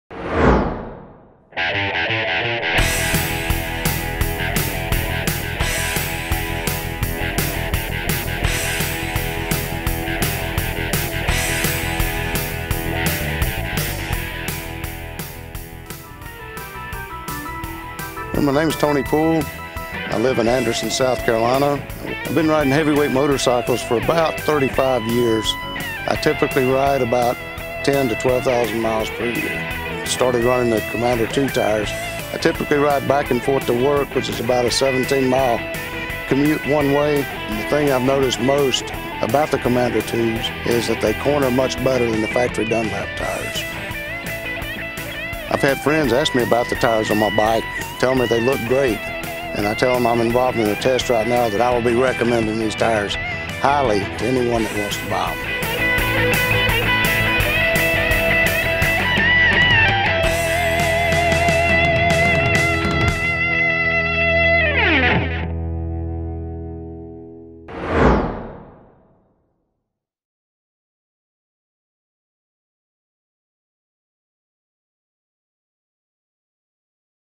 Michelin Commander II Testimonial - Tony Poole • Shop J&P Cycles
Tony Poole talks about his experience with the Michelin Commander II series tires available at J&P Cycles. Tony uses the Commander II tires for his daily commute and is happy with how they corner.